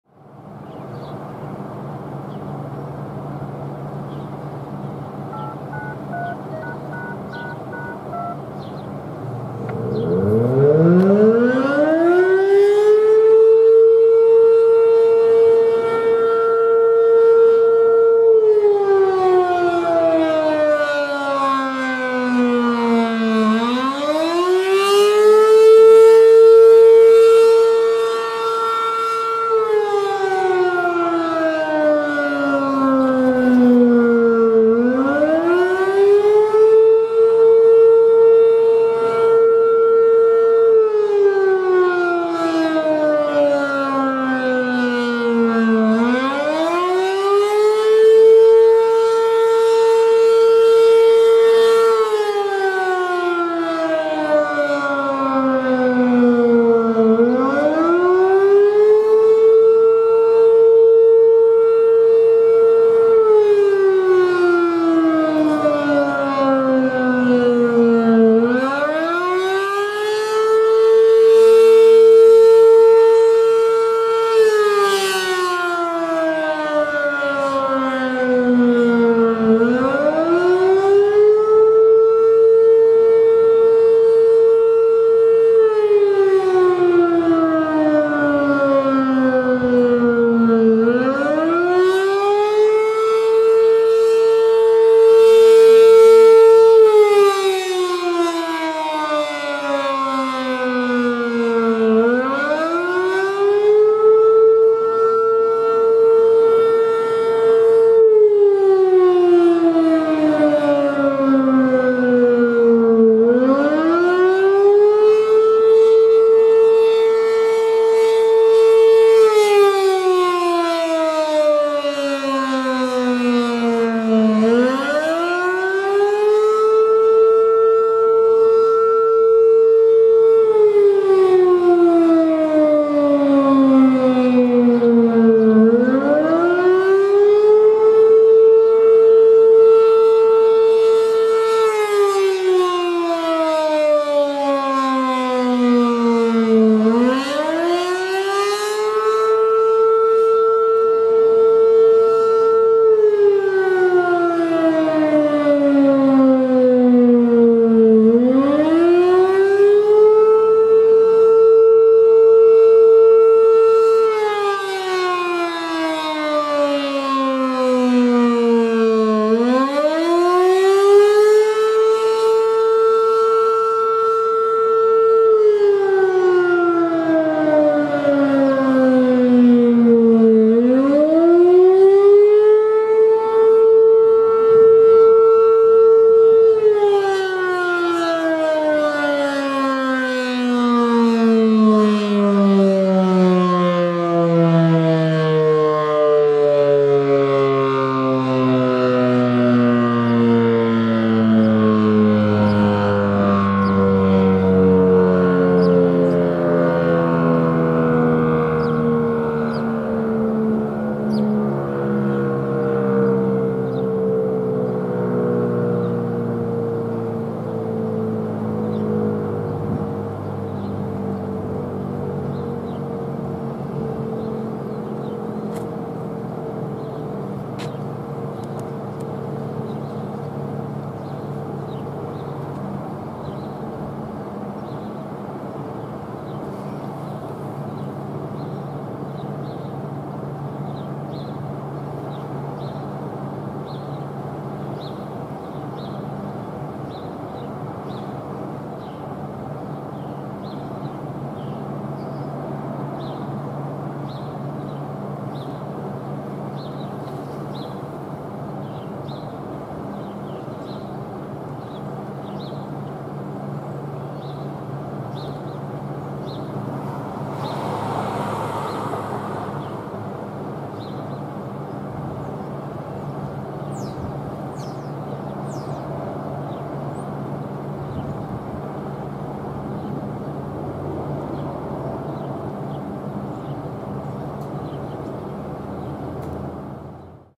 ACA P-10 Siren Test - Salem, WI - Full Attack - 4/20/24
Getting lucky on 4/20

So now that I've gotten videos of everything in the county (excluding Wilmot since it's broken), I'm just going around recording everything in the opposite cycle. And I'm going in order of when I recorded them, starting with this single tone P-10. This was a pleasant surprise today as normally Kenosha goes pretty short. You don't see me go under the siren during the secondary since I had been standing by my cameras for a decent portion of the test waiting for it wind down, and not expecting full attack. Still, this was really cool and I'm actually curious to know if any other enthusiasts were in the county when this happened, as I'm sure it wasn't a single siren fluking.

Location: Village of Salem Lakes Fire/Rescue Station 2
Replaced: B&N Mobil Directo BN44E
Testing Time: Third Saturday at 10:30AM
Testing Cycle: Full Attack
Enthusiasts Present: N/A
Main Shot: Canon Powershot SX540 HS
Secondary Shot & Audio: Panasonic HC-V180K
County: Kenosha